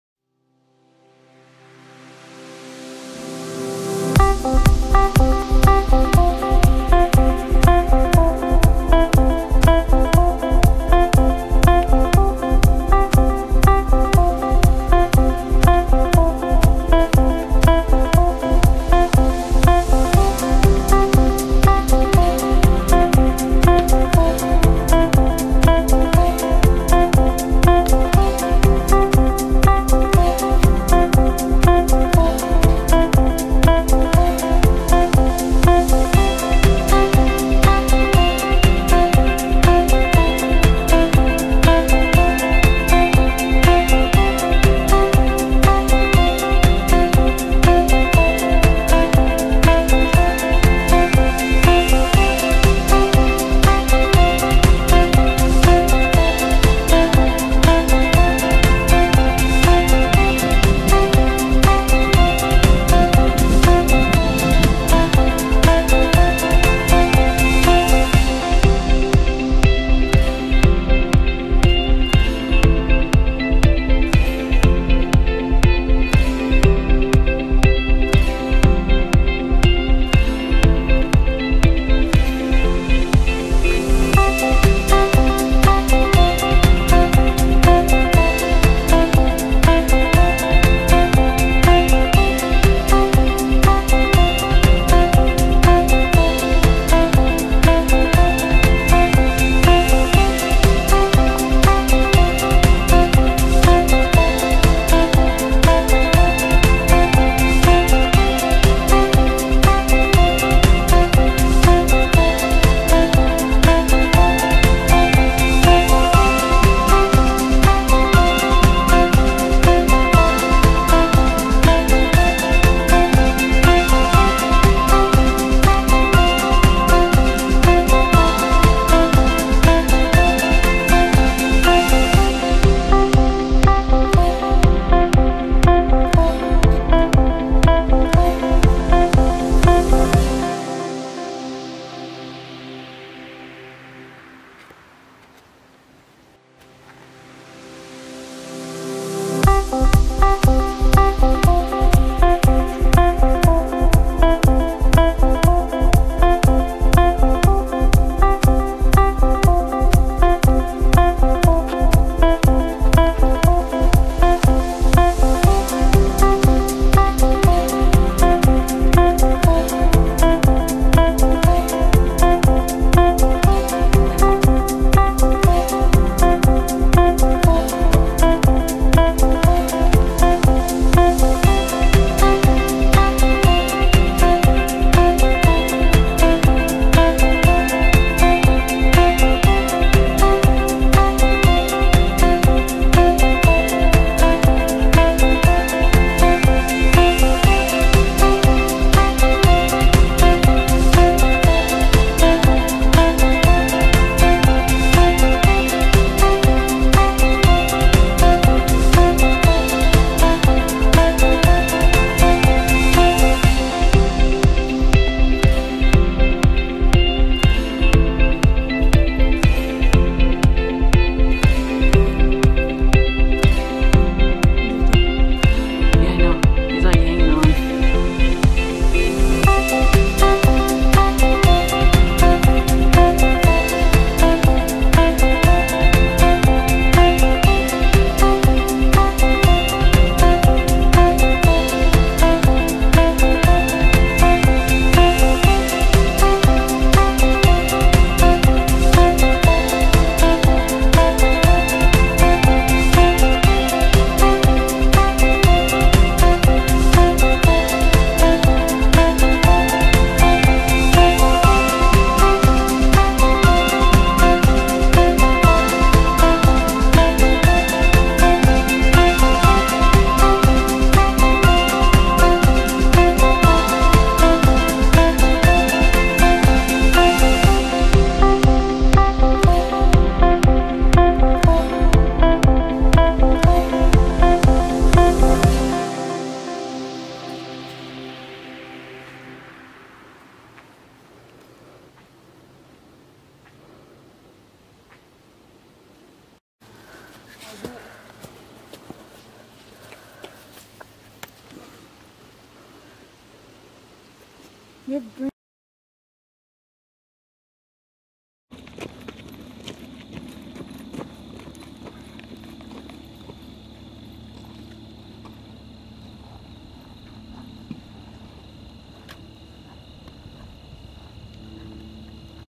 NOOK NISHVENTURES Episode One
My first vid.  We are an indigenous fam in Northern Ontario, just sharing our respect and love for Mother Earth.  NOOK is the doggo!  We are Seven Wolves